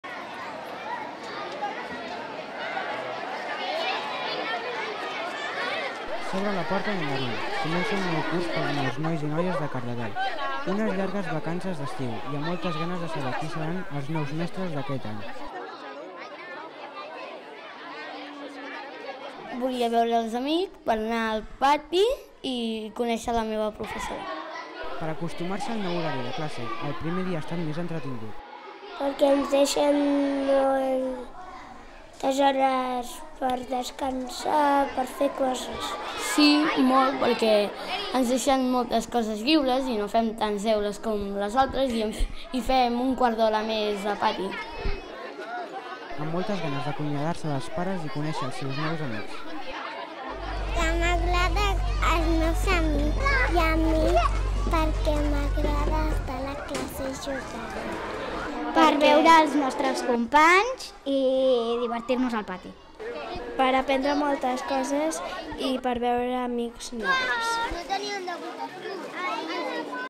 Torrnada a l'escola, tornada a la rutina
Molts nois i noies de Cardedeu tornen a la rutina habitual però... com ha estat el primer dia? tenien ganes de trobar-se amb els amics? i amb les mestres?